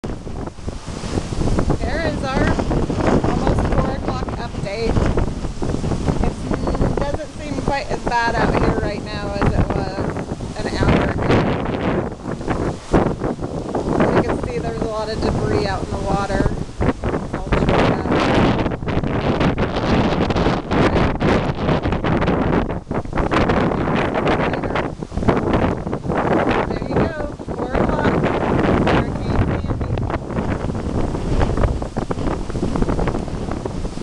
Hurricane Sandy 4pm Montego Bay Jamaica
Aubree's Travel inside the storm at Secrets St. James